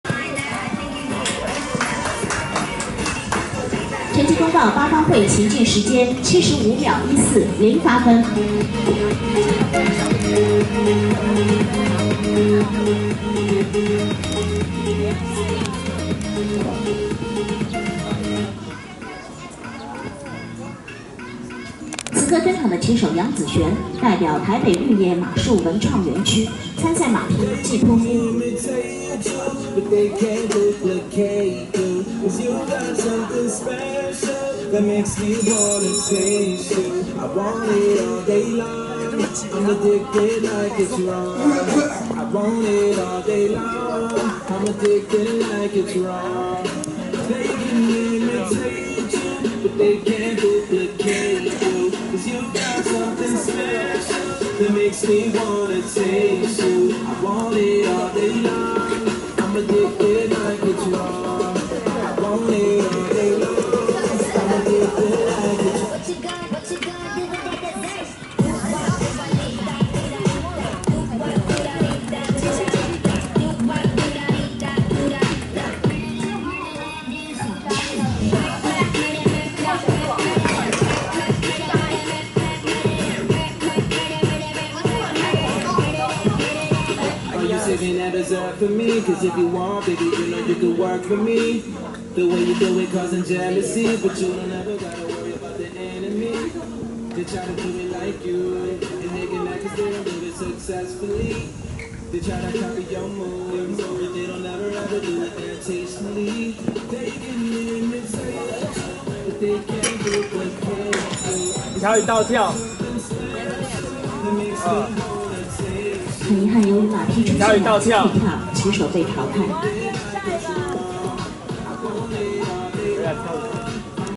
2018第五屆海峽盃馬術錦標賽團體賽(騎手:楊芷璇 馬匹:季托夫)
2018第五屆海峽盃馬術錦標賽團體賽(騎手:楊芷璇 馬匹:季托夫)